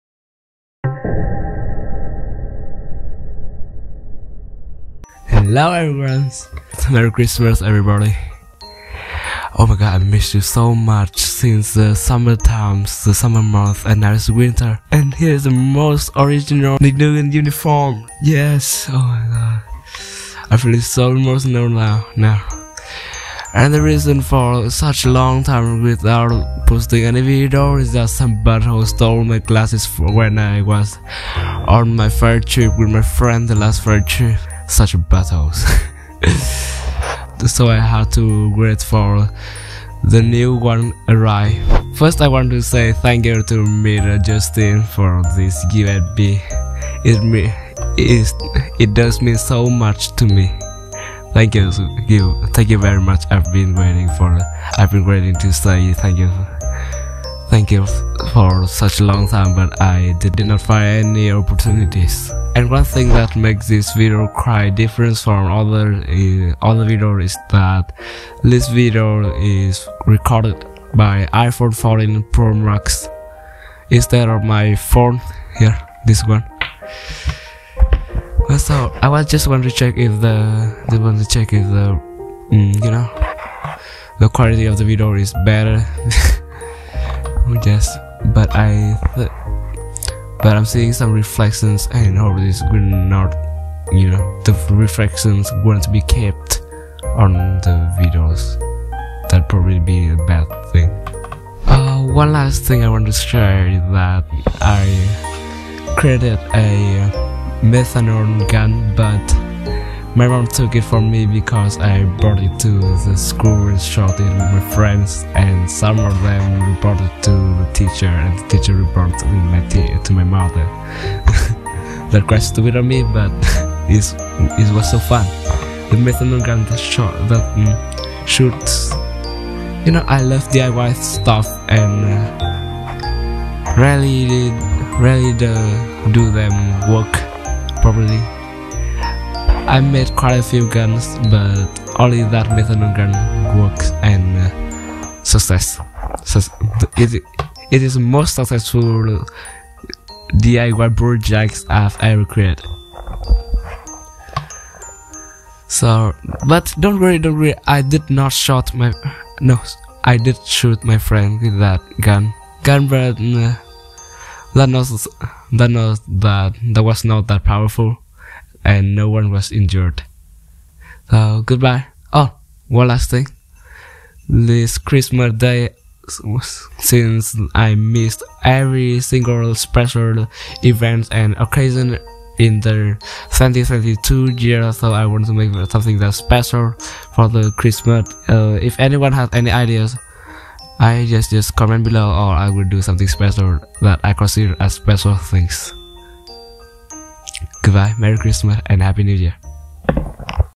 [Nig'sDiary #13] Some BUTTHOLES stole my glasses : ) Merry Xmas! | Nig Nugen
PLEASE DO NOT SUBCRISBE !!
It's so emotional to sit here talking to nobody out there. I don't know when but recording myself is a weird hobby in which I really interested! Always feel nice to come back, huh? It's Christmas eve today so happy Christmas my friends.